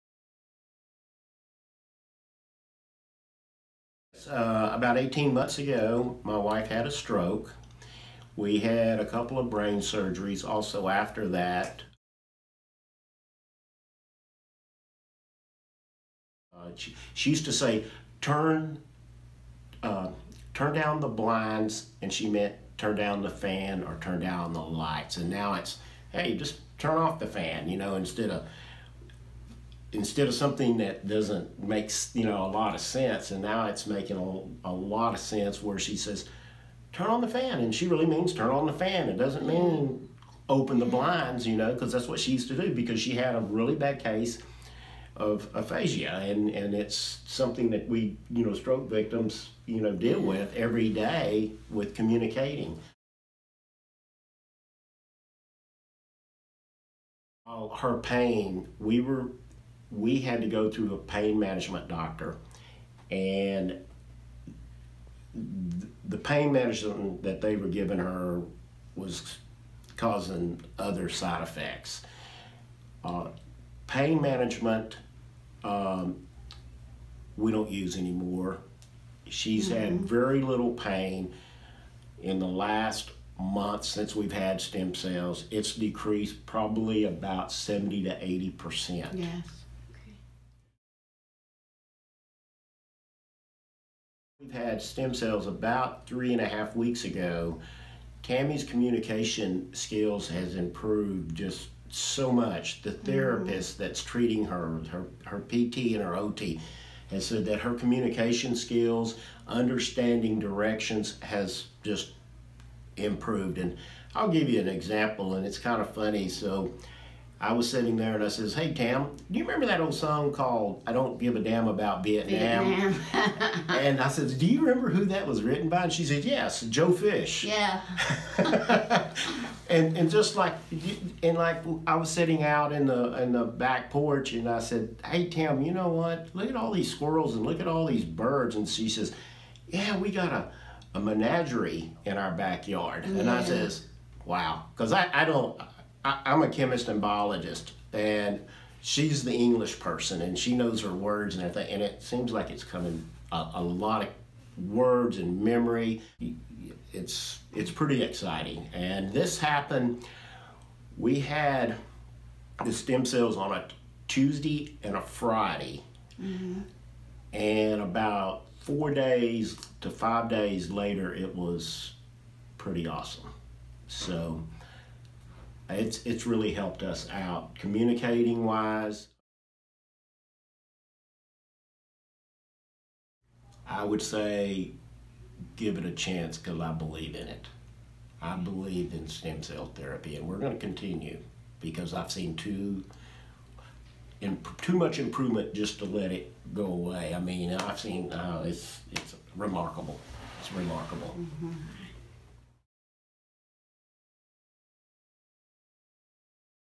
Stroke Treatment with Stem Cell Therapy
Mrs. Broke had stroke 18 months ago and suffered from Broca's aphasia, poor memory, and pain. Within weeks after stem cell therapy, she started making full sentences, her memory improved dramatically and got off all her pain medications.